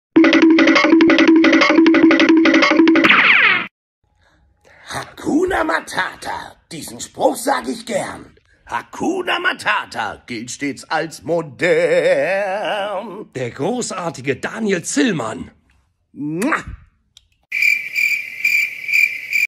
Peter trifft Synchronsprecher: Daniel Zillmann (Pumbaa in Der König der Löwen (2019))
PUMBAA in Der König der Löwen (2019)